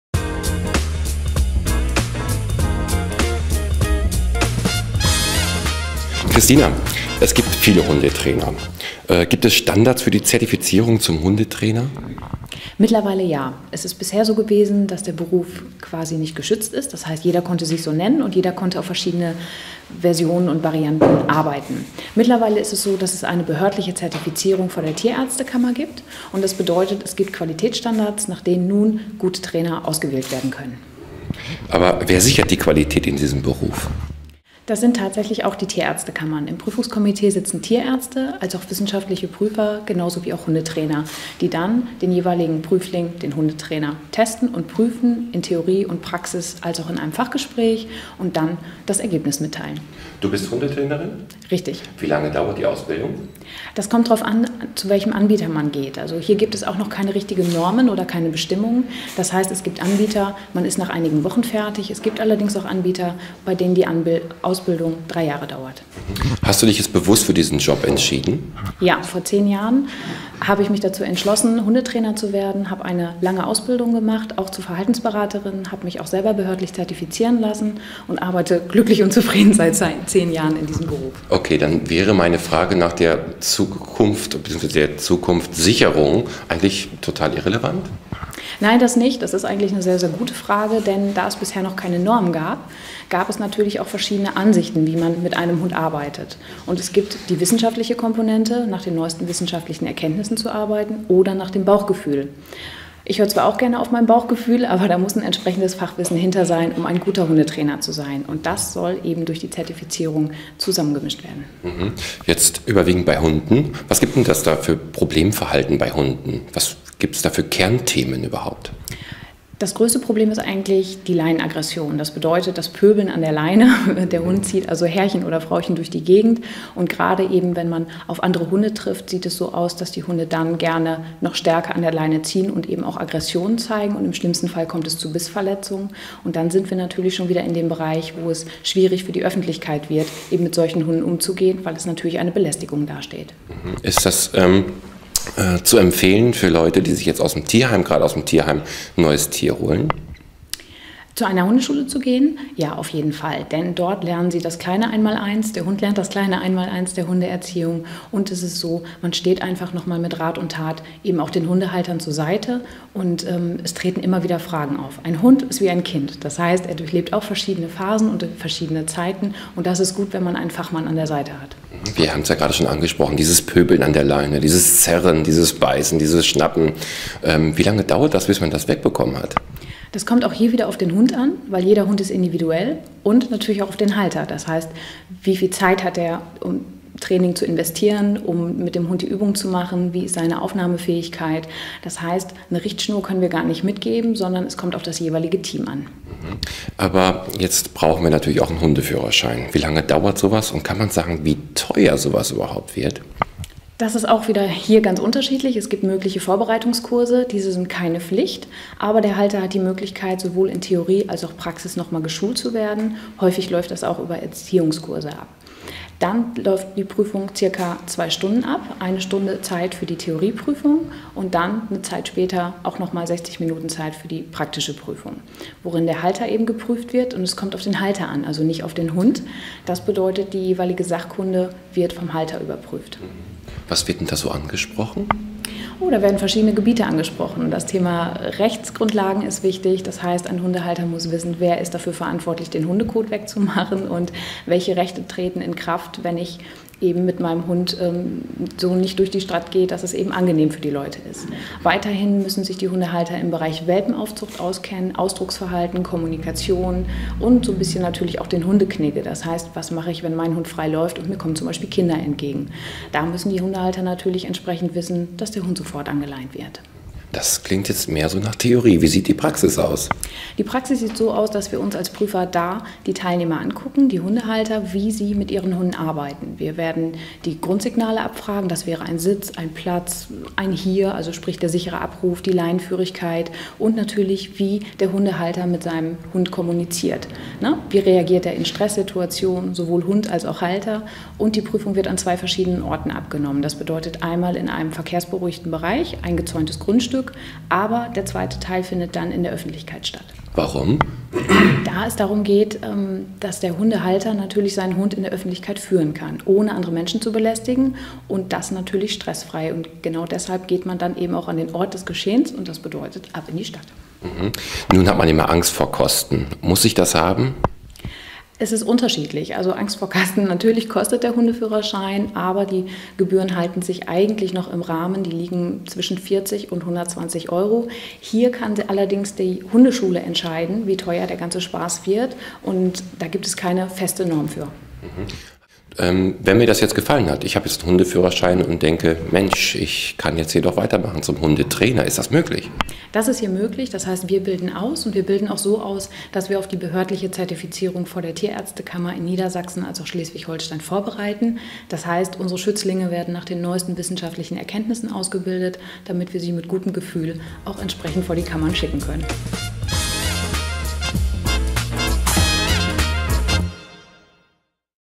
Ziemer und Falke - Hundeschule - Hundeführerschein - Hundetrainer
Der richtige Umgang mit Tieren ist ebenso wichtig, wie der richtige UMgang mit unserer Umwelt. Als Teamplayer spielen Hund und Besitzer eine gemeinsame Rolle bei der Ausbildung zum Hundeführerschein. Mehr noch: Wer lust auf mehr hat, dem steht das Ziel zum Hundetrainer nichts mehr im Wege. Kristina Falke erklärt im Inteview Abläufe, Umgang und Wege.